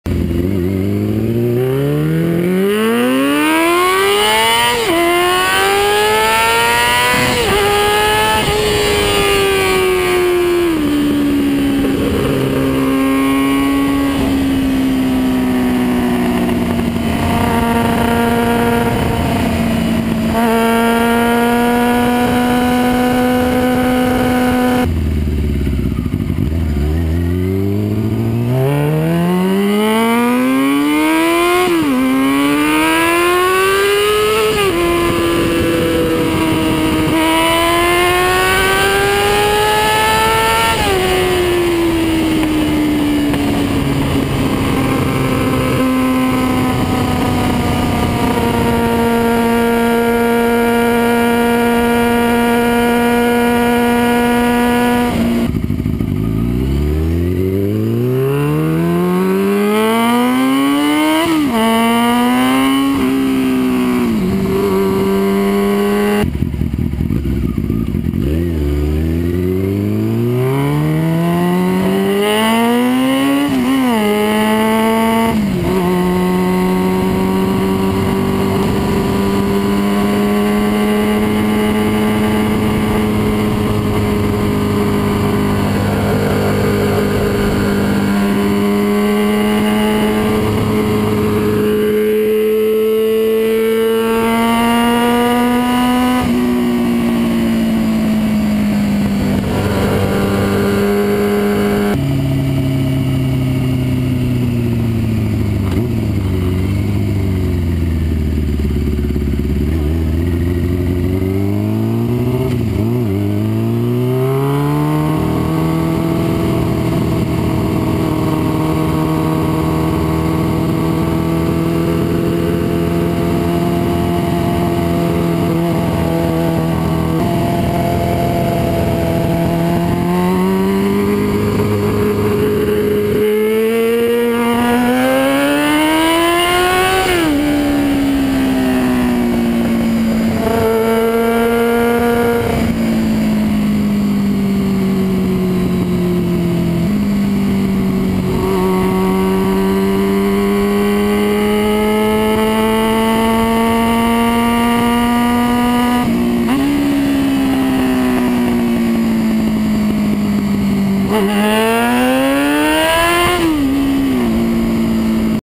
HR1 Riot Sound clip 2002 Yamaha R6
Video for people who wanted sound clips of the exhaust. HR1 Riot slip-on 2002 Yamaha R6